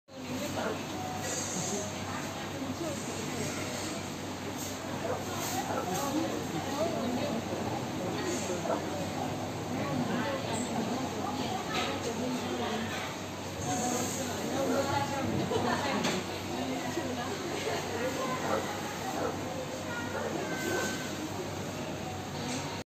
Old English Sheepdog barking | Cannot wait for playtime
Old English Sheepdogs waiting to go out for playtime!